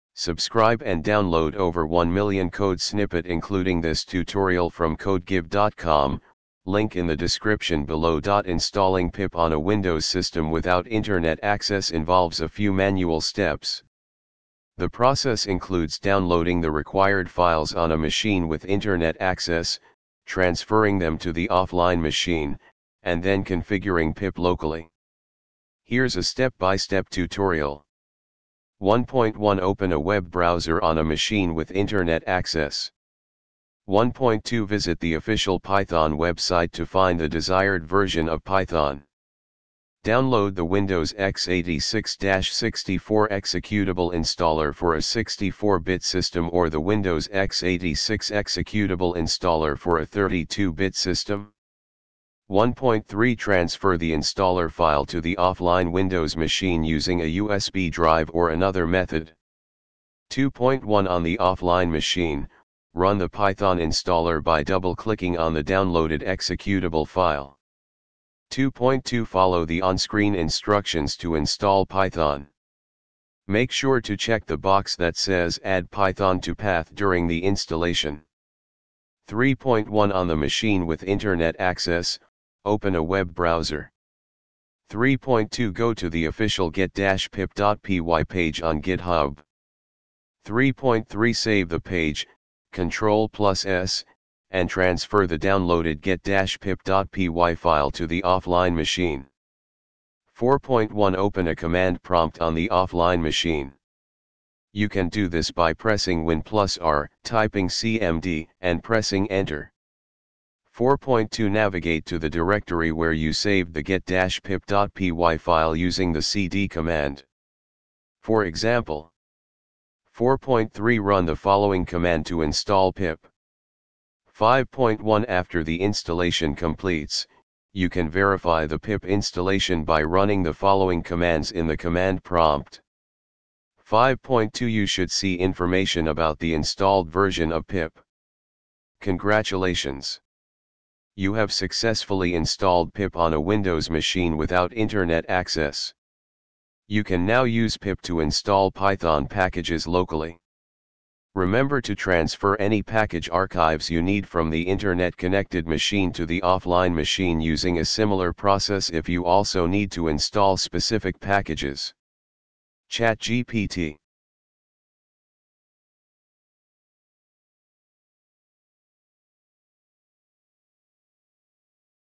how to install pip on windows without internet
Installing pip on a Windows system without internet access involves a few manual steps. The process includes downloading the required files on a machine with internet access, transferring them to the offline machine, and then configuring pip locally. Here's a step-by-step tutorial:
1.1 Open a web browser on a machine with internet access.
1.2 Visit the official Python website to find the desired version of Python. Download the Windows x86-64 executable installer for a 64-bit system or the Windows x86 executable installer for a 32-bit system.
1.3 Transfer the installer file to the offline Windows machine using a USB drive or another method.
2.1 On the offline machine, run the Python installer by double-clicking on the downloaded executable file.
2.2 Follow the on-screen instructions to install Python. Make sure to check the box that says "Add Python to PATH" during the installation.
3.1 On the machine with internet access, open a web browser.
3.2 Go to the official get-pip.py page on GitHub.
3.3 Save the page (Ctrl + S) and transfer the downloaded get-pip.py file to the offline machine.
4.1 Open a command prompt on the offline machine. You can do this by pressing Win + R, typing cmd, and pressing Enter.
4.2 Navigate to the directory where you saved the get-pip.py file using the cd command. For example:
4.3 Run the following command to install pip:
5.1 After the installation completes, you can verify the pip installation by running the following commands in the command prompt:
5.2 You should see information about the installed version of pip.
Congratulations! You have successfully installed pip on a Windows machine without internet access. You can now use pip to install Python packages locally. Remember to transfer any package archives you need from the internet-connected machine to the offline machine using a similar process if you also need to install specific packages.
ChatGPT